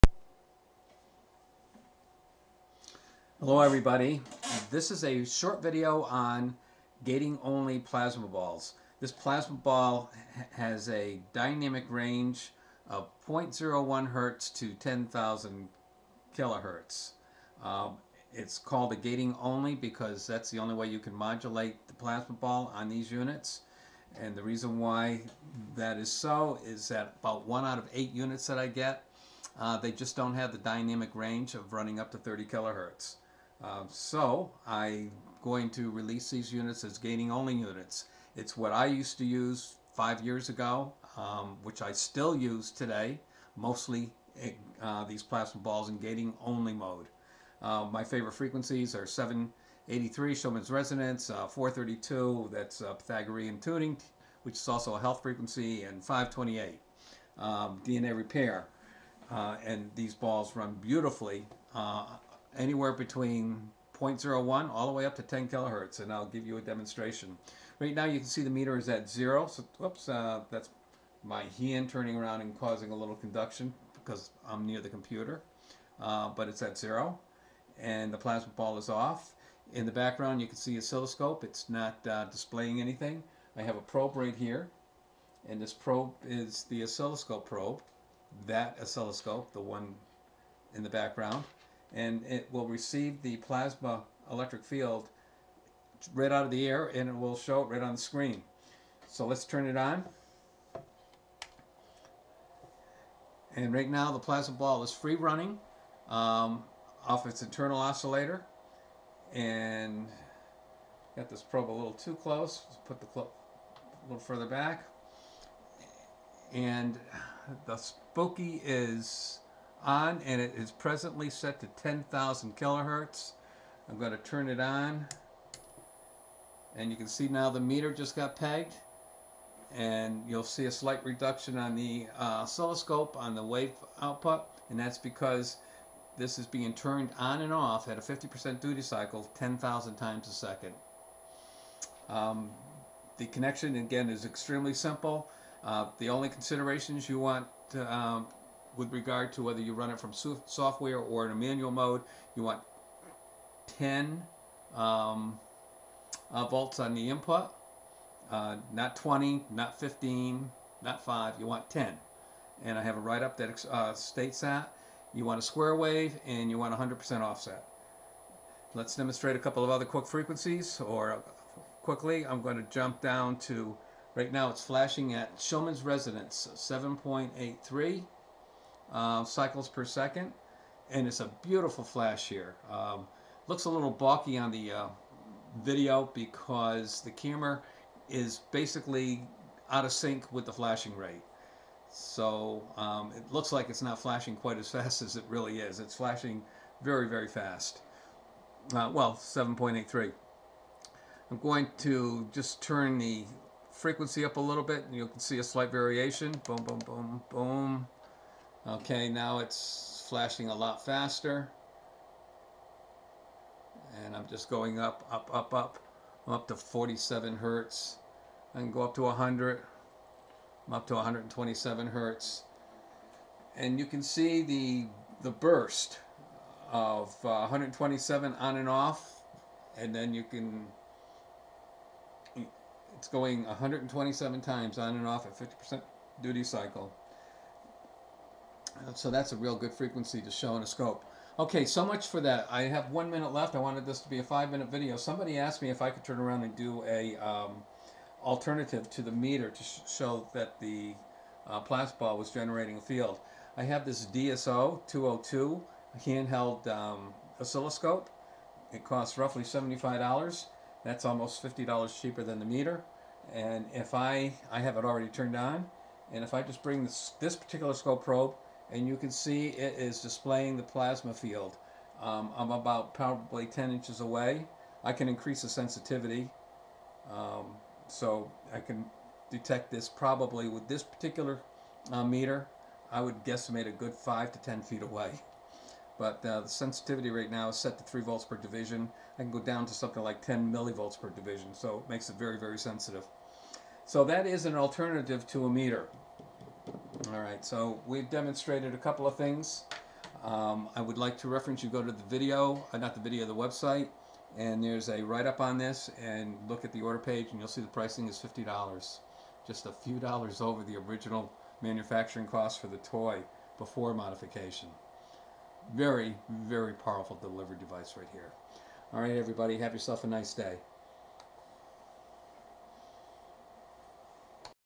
.01Hz to 10K Gating Only Plasma Ball
Modulating a Plasma Ball through gating.  Gating is the actual turning on and off of the Plasma Ball a specific number of times.  The frequency dialed in on a function generator like the Spooky2 represents the number of times the Plasma Ball is flashed on and off.  This is an extremely powerful Rife technique for delivering an Electric Field at specific health modality frequencies.  for more information.  Respectfully and be well, Steve